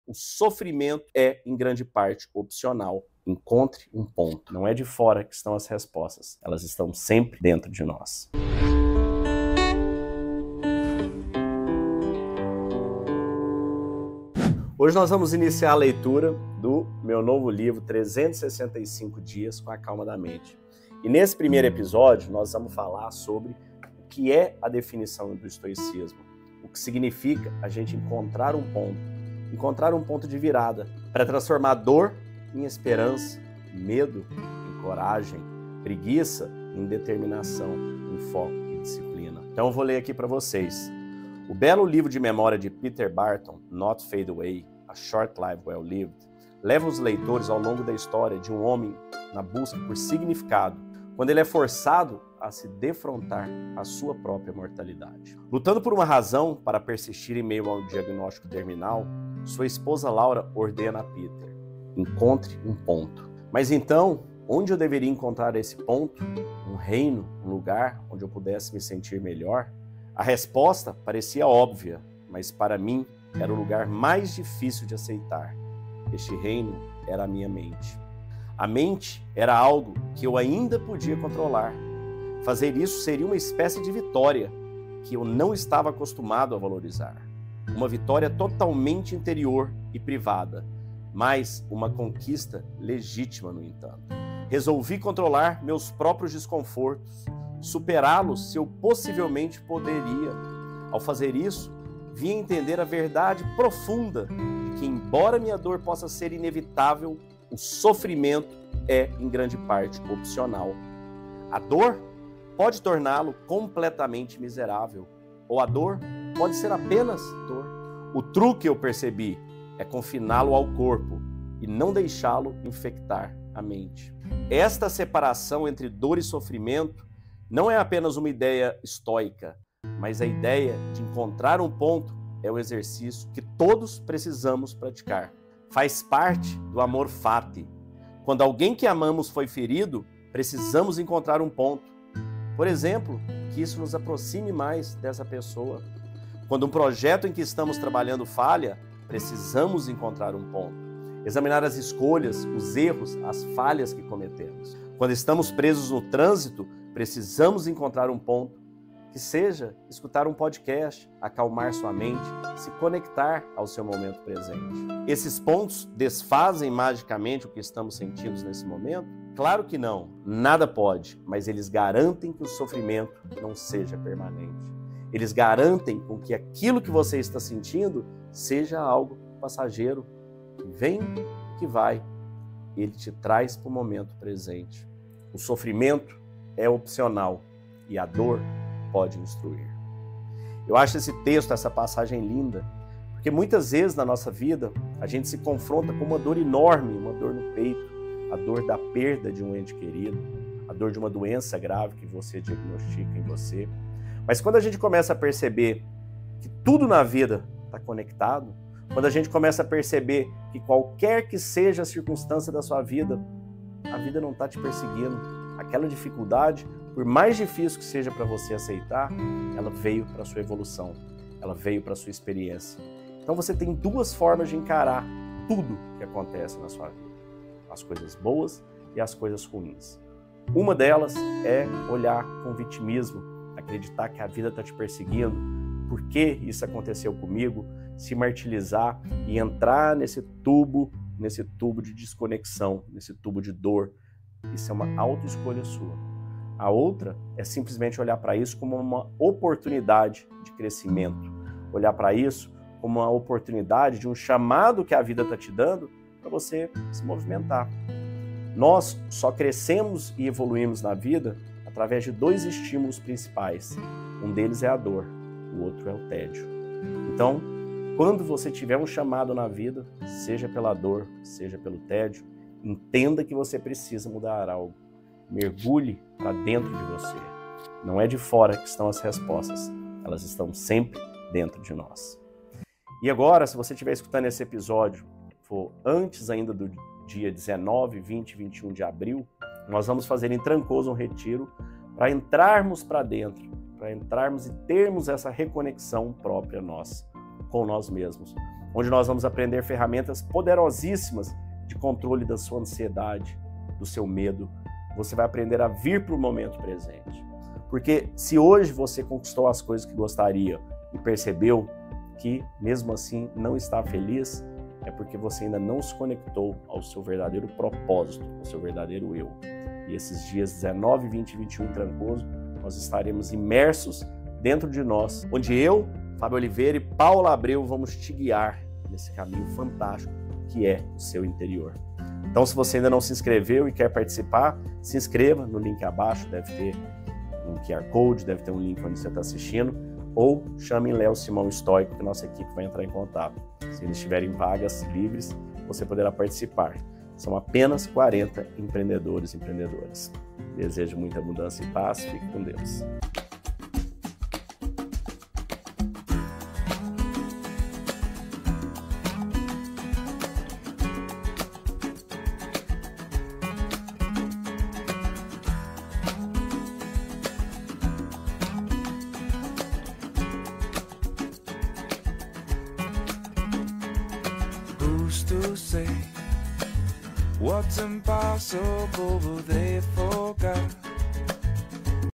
Encontre um ponto e transforme a sua dor #795
Trancoso Fora da Caixa 2024 - Uma imersão transformadora para líderes de alta performance. Clique e saiba mais

Encontre um ponto e transforme a sua dor

Hoje nós vamos iniciar a leitura do meu novo livro '365 dias com a calma da mente', e nesse primeiro episódio nós vamos falar sobre o que é a definição do estoicismo.

O que significa a gente encontrar um ponto, encontrar um ponto de virada para transformar dor em esperança, medo em coragem, preguiça em determinação, um foco e disciplina. Então eu vou ler aqui para vocês o belo livro de memórias de Peter Barton, 'Not Fade Away: A Short Life Well Lived', leva os leitores ao longo da história de um homem na busca por significado quando ele é forçado a se defrontar com a sua própria mortalidade, lutando por uma razão para persistir em meio ao diagnóstico terminal.

Sua esposa, Laura, ordena a Peter: 'encontre um ponto'. "Mas então, onde eu deveria encontrar esse ponto? Um reino, um lugar onde eu pudesse me sentir melhor. A resposta parecia óbvia, mas para mim era o lugar mais difícil de aceitar. Este reino era a minha mente. A mente era algo que eu ainda podia controlar. Fazer isso seria uma espécie de vitória que eu não estava acostumado a valorizar, uma vitória totalmente interior e privada, mas uma conquista legítima. No entanto, resolvi controlar meus próprios desconfortos, superá-los se eu possivelmente pudesse. Ao fazer isso, vim a entender a verdade profunda de que, embora minha dor possa ser inevitável, o sofrimento é em grande parte opcional. A dor pode torná-lo completamente miserável, ou a dor pode ser apenas dor. O truque, eu percebi, é confiná-los.

Precisamos praticar, faz parte do amor, quando alguém que amamos foi ferido precisamos encontrar um ponto, por exemplo, que isso nos aproxime mais dessa pessoa. Quando um projeto em que estamos trabalhando falha, precisamos encontrar um ponto, examinar as escolhas, os erros, as falhas que cometemos. Quando estamos presos no trânsito, precisamos encontrar um ponto, que seja escutar um podcast, acalmar sua mente, se conectar ao seu momento presente. Esses pontos desfazem magicamente o que estamos sentindo nesse momento. Claro que não, nada pode, mas eles garantem que o sofrimento não seja permanente. Eles garantem que aquilo que você está sentindo seja algo passageiro, que ele te traz momento presente. O sofrimento é opcional e a dor pode instruir. "

Eu acho esse texto, essa passagem linda, porque muitas vezes na nossa vida, a gente se confronta com uma dor enorme, uma dor no peito, a dor da perda de um ente querido, a dor de uma doença grave que você diagnostica em você. Quando a gente a perceber na vida, conectar perceber que, qualquer que seja a circunstância da sua vida, a vida não tá te perseguindo, aquela dificuldade, por mais difícil que seja para você aceitar, ela veio pra sua evolução, ela veio pra sua experiência. Então você tem duas formas de encarar tudo que acontece na sua vida, as coisas boas e as coisas ruins.

Uma delas é olhar com vitimismo, acreditar que a vida tá te perseguindo, por isso aconteceu comigo. Se martirizar e entrar nesse tubo, nesse tubo de desconexão, nesse tubo de dor. Isso é uma auto escolha sua. A outra é simplesmente olhar para isso como uma oportunidade de crescimento, olhar para isso como uma oportunidade de um chamado que a vida tá te dando para você se movimentar. Nós só crescemos e evoluímos na vida através de dois estímulos principais, um deles é a dor, o outro é o tédio.

Então quando você tiver um chamado na vida, seja pela dor, seja pelo tédio, entenda que você precisa mudar algo. Mergulhe para dentro de você, não é de fora que estão as respostas, elas estão sempre dentro de nós.

Venha se juntar a nós nesta jornada de reflexão e crescimento! No episódio de estreia do nosso novo livro "365 Dias com a Calma da Mente", exploramos os conceitos profundos do estoicismo e como podemos encontrar pontos de virada para transformar desafios em oportunidades de crescimento. Com histórias comoventes e insights inspiradores, este episódio é uma jornada de autoconhecimento e superação.

Não perca a chance de se conectar com a mensagem poderosa deste episódio e compartilhá-la com aqueles que você ama. Junte-se a nós e vamos juntos aprender a transformar a dor em esperança, o medo em coragem e a preguiça em determinação. Curta, compartilhe e faça parte deste movimento de crescimento pessoal!